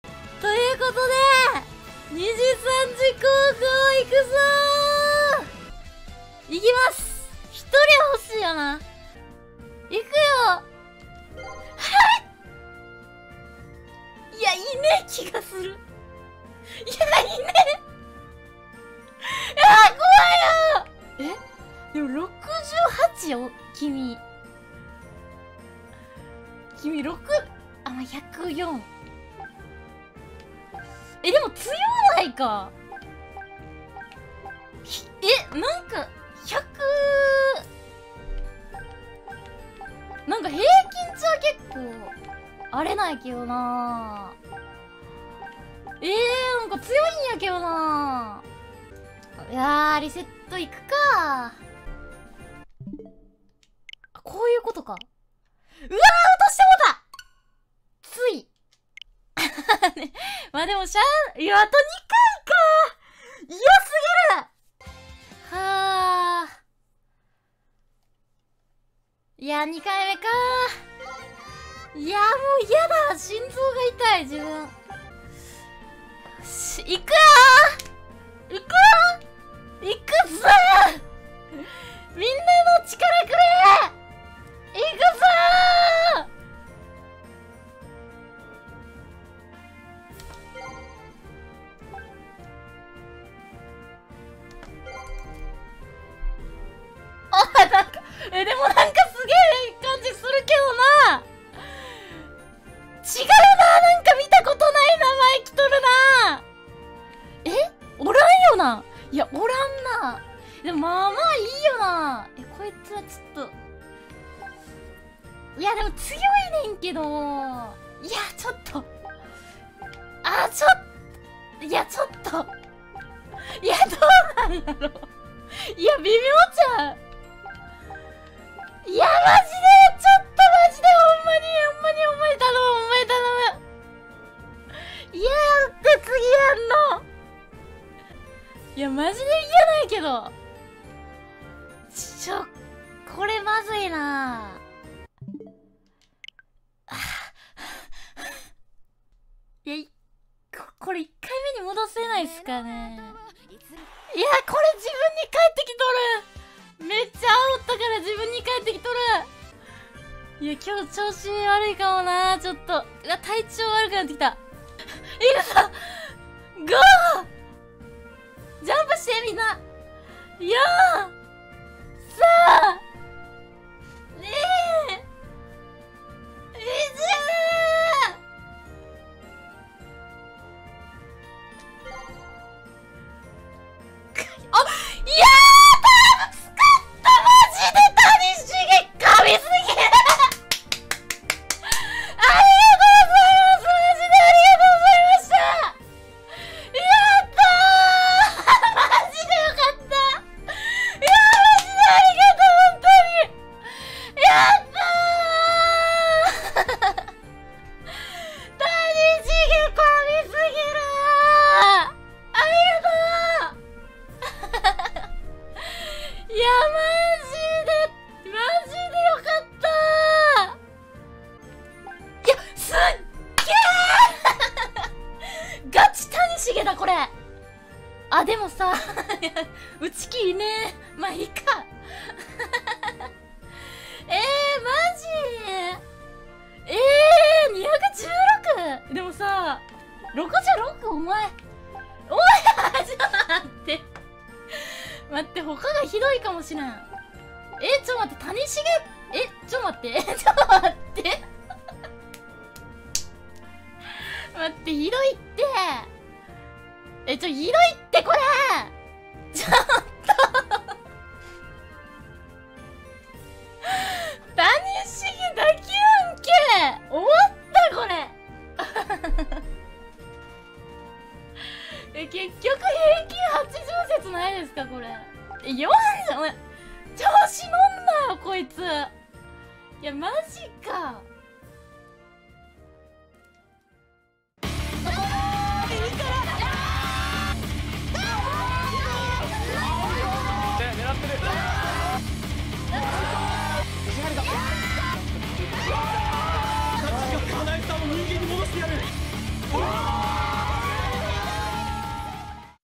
物凄いプレッシャーの中、３度目の正直を見せる豪運しぃしぃ（椎名唯華）
元動画
【にじさんじ甲子園】にじさんじ高校2022！！IKZ！！！【椎名唯華/にじさんじ】

Ａリーグ：2022年8月12日(金) 13:00～18:00(予定)
Ｂリーグ：2022年8月13日(土) 13:00～18:00(予定)
　決勝　：2022年8月14日(日) 18:00～22:30(予定)

魔王魂

ホロライブ切り抜き登録済です